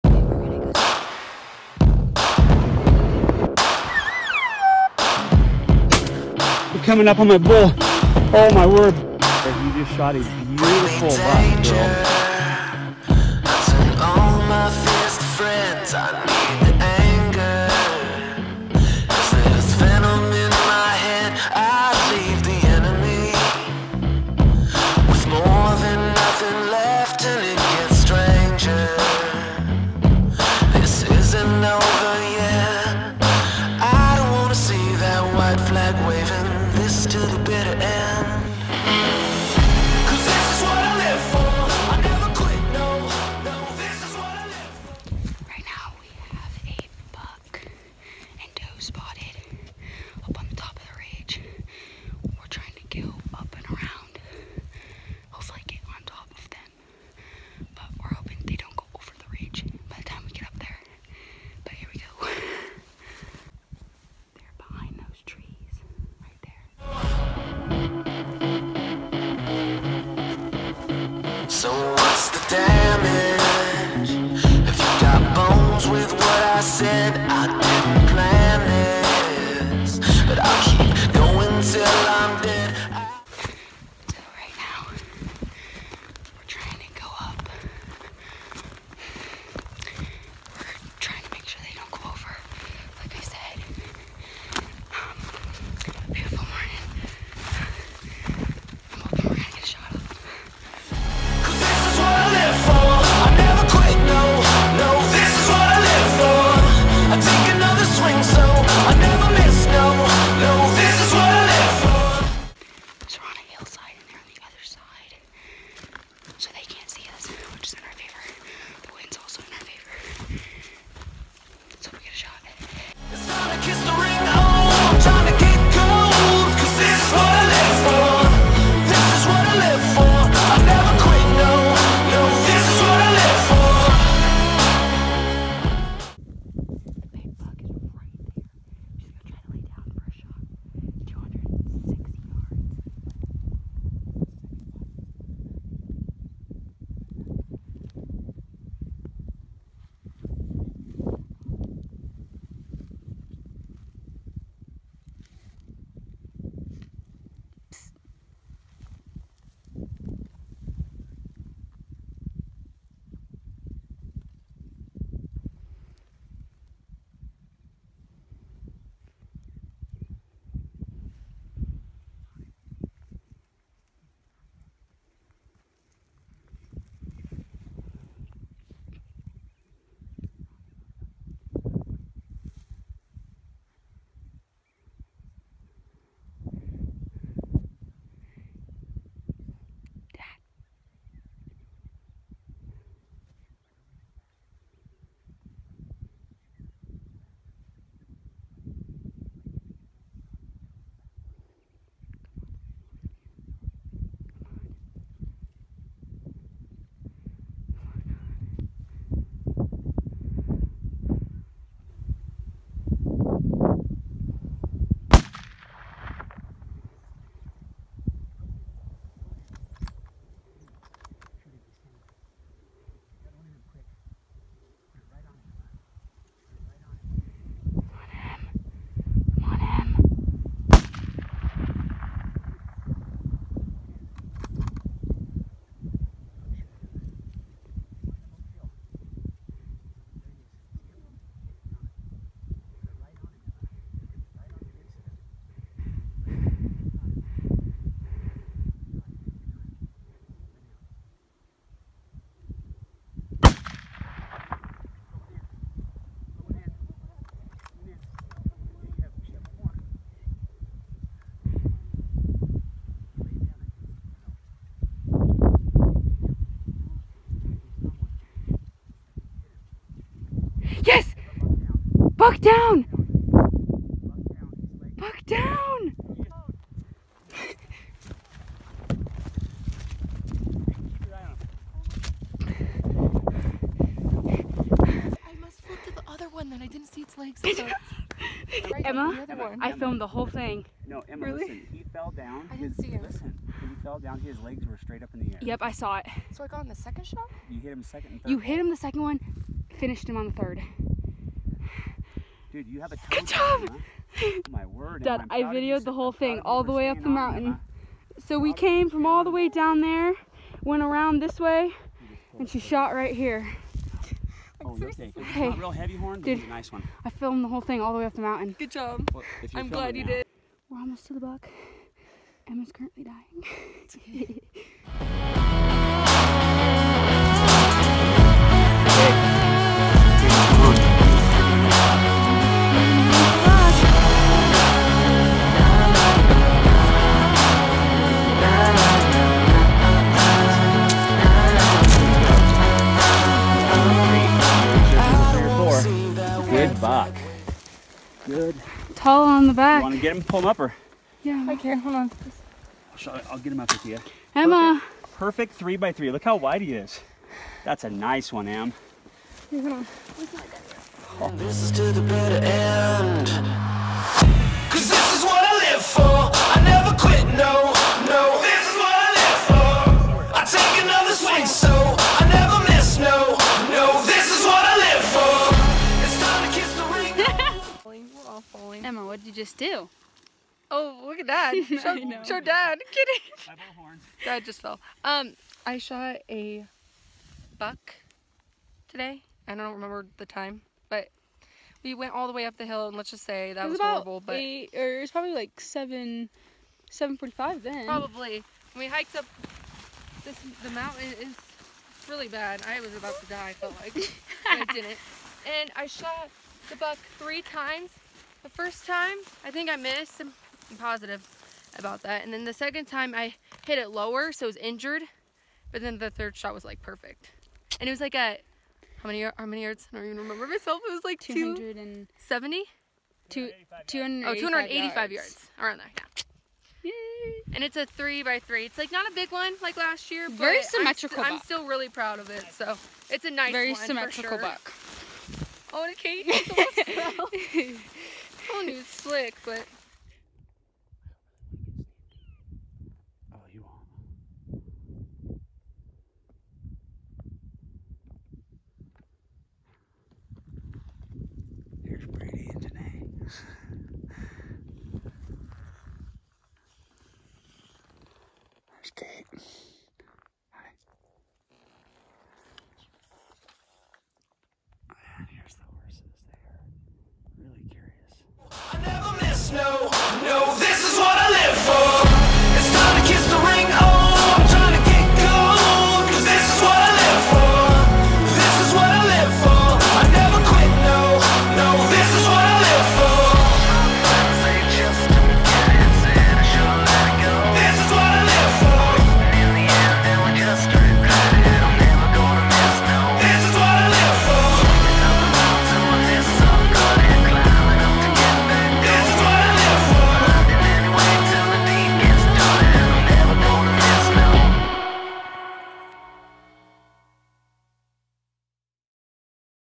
Throwback Eastern Oregon Youth Mule Deer Hunt | Short Film
Throwback Eastern Oregon Youth Mule Deer Hunt. Enjoy this short film of Emma taking a nice Oregon mule deer.

PERSISTENCE EQUALS SUCCESS.